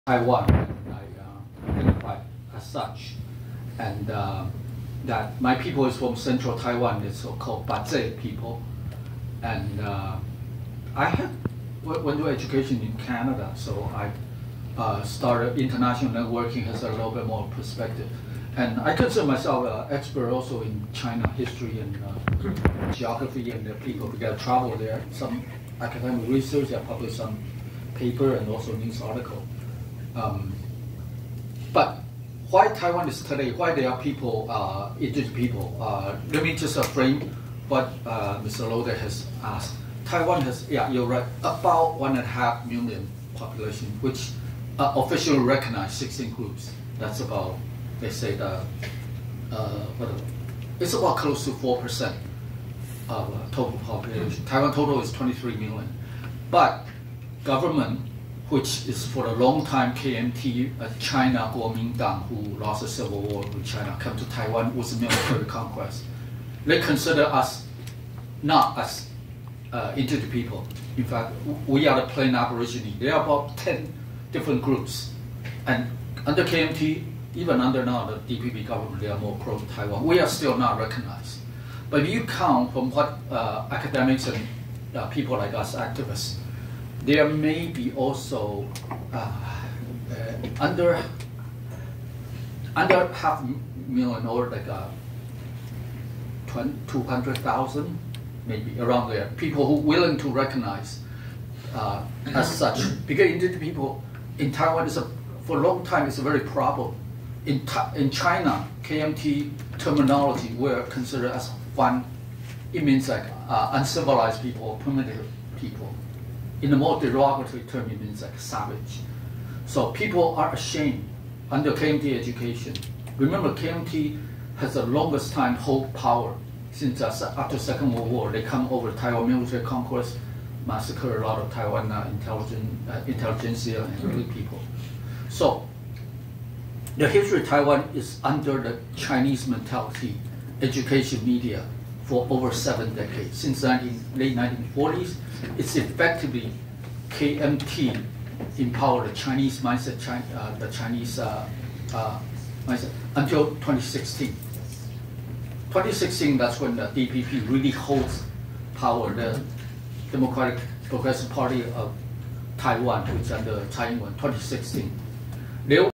Jason Pan Adawai Free Nations PostRussia Forum Design of post-Russian spaces Taiwan Taipei 18.09.24
Jason Pan Adawai (Deputy Director of taiwan indigenous rights group, former Executive Council member of Thailand-based, IP rights and international networking organization AIPP) fragment of speech at 12th Free Nations postrussia Forum Design of postrussianspaces and its impact on the macro-regions of East, Southeast & Central

 Discussion Panel 4: Fostering global solidarity: Free post-Russia nations & Indigenous peoples of Taiwan and the broader Asia-Pacific region
Moderator: Oliver Loode, Managing Director of URALIC Centre (Estonia), former member of the United Nations Permanent Forum on Indigenous Issues (UNPFII)
Speakers
Marina Khankhalaeva, Founder and leader of Tusgaar Buryad-Mongolia movement, a founding member of the buryatia Independence Committee
Dr. Jolan HSIEH, Professor and Director, Center for International Indigenous Affairs at National Dong Hwa University, President, Taiwan Security Institute
Jun Tanaka, Political activist, Writer
Jason Pan Adawai, Deputy Director of Taiwan indigenous rights group, former Executive Council member of Thailand-based, IP rights and international networking organization AIPP.